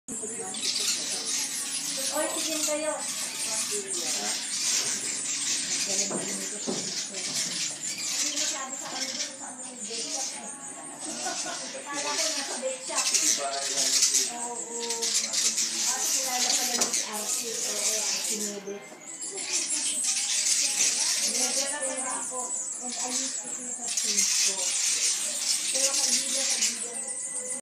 Balinsasayaw nests inside an old house in Carigara, Leyte
On September 21, 2013, we passed by Carigara, Leyte on our way to conduct an event in Ormoc. Our speaker wanted to buy Roscas, a type of cookie shaped like a boomerang. We went to this old rustic house in Carigara and was surprised to find not only Roscas and carabao milk pastillas (Lumen's Pastillas de Leche) but also Balinsasayaw or edible-nest swiftlets dancing around their living room.

These birds are famous for their birds' nest (made out of their saliva) which is used in preparing the expensive birds' nest soup.